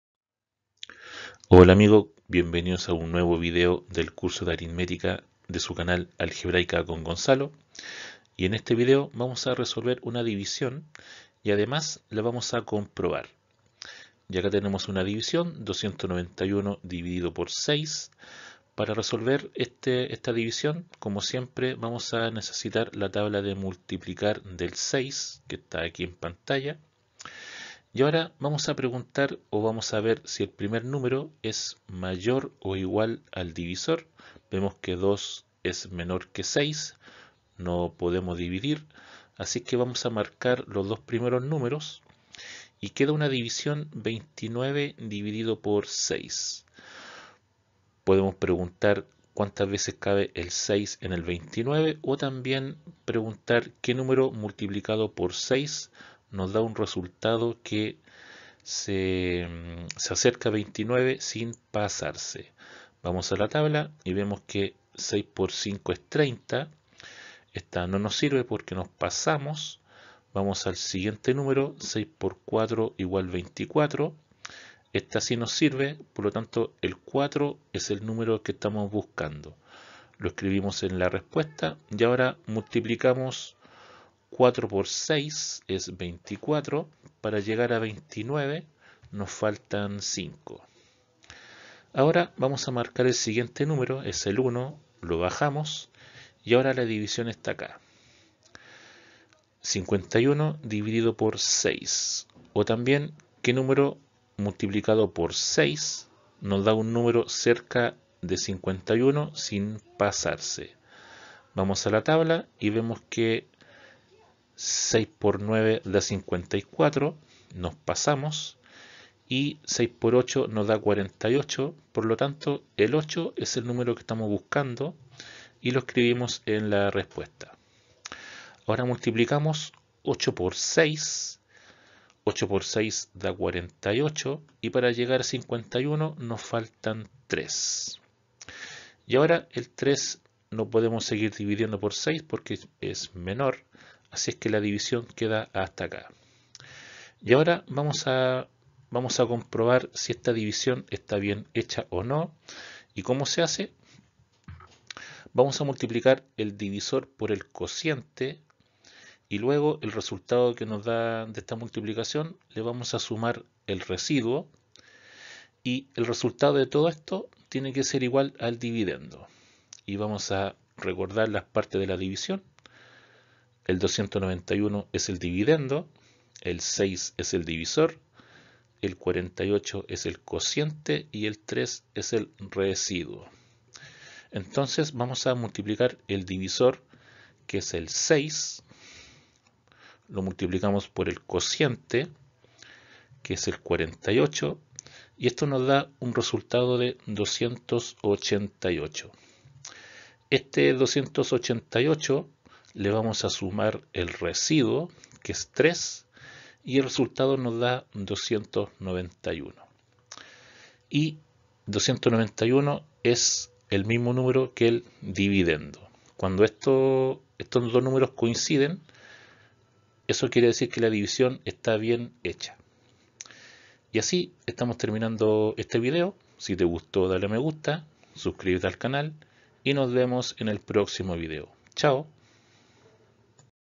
Curso de Aritmética: video 71. División y comprobación. E1.
👉 En este video vamos a resolver el primer ejemplo de una división y además la vamos comprobar si la división está bien resuelta.

👏 👏 👏 👏 👏 😎 😎          DONACIONES         😎  😎  👏 👏 👏 👏 👏